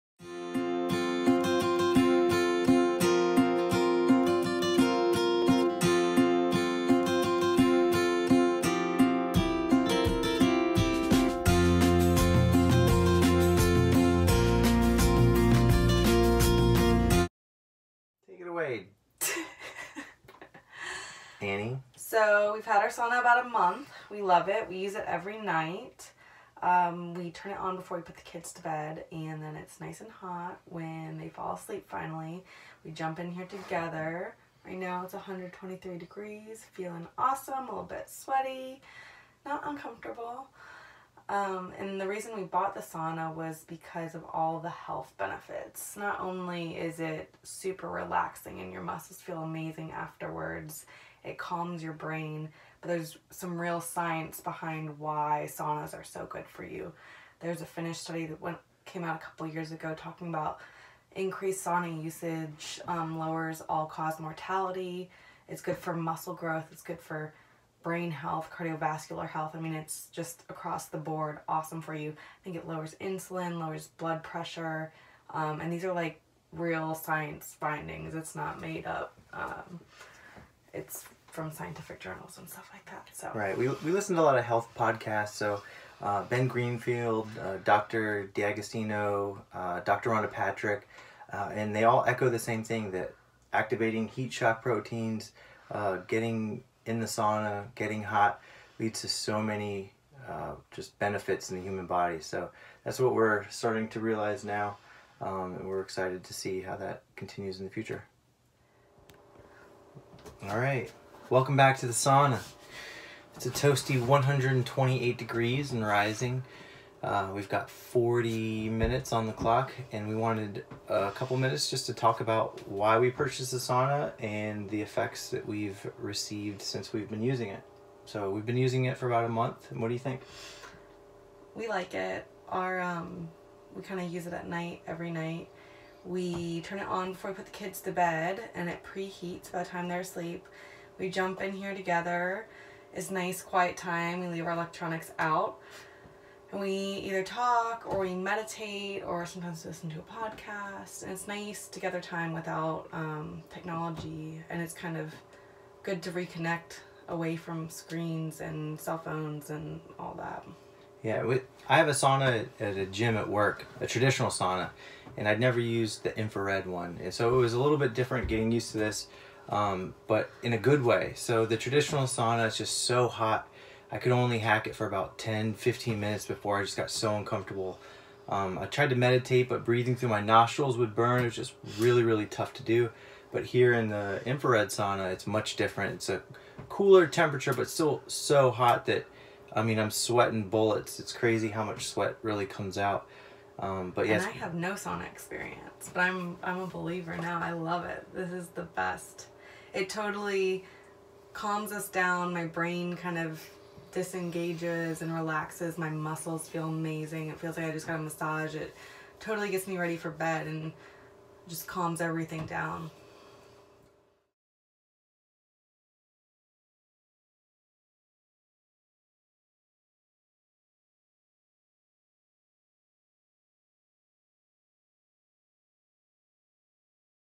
Jared and Anne Love Their Jacuzzi® Infrared Sauna
Jared and Anne talk about the benefits they've received from using their sauna and why they chose a Clearlight Premier IS-C.  They also show a time-lapse video of the 1-hour installation.